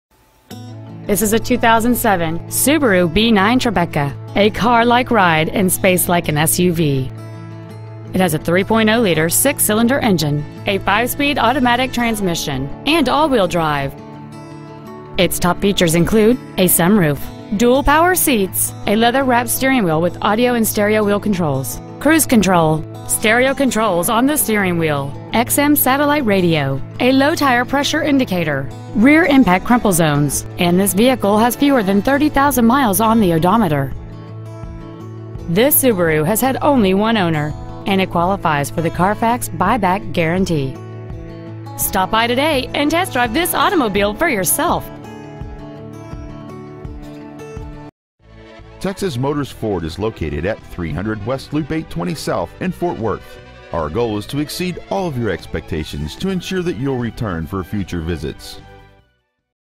2007 Subaru B9 Tribeca Fort Worth TX
We are proud to present this 2007 Subaru B9 Tribeca.

 Year: 2007
 Make: Subaru
 Model: B9 Tribeca
 Engine: Gas 6-cyl 3.0L/183
 Trans.: Automatic
 Exterior: Titanium Silver Metallic
 Miles: 29,795
 Interior: Slate Gray

 300 West Loop 820 South

Welcome to Texas Motors Ford. Come test drive a Ford. We offer Dallas-Fort Worth top selection of Ford vehicles.

 Pre-Owned 2007 Subaru B9 Tribeca Fort Worth TX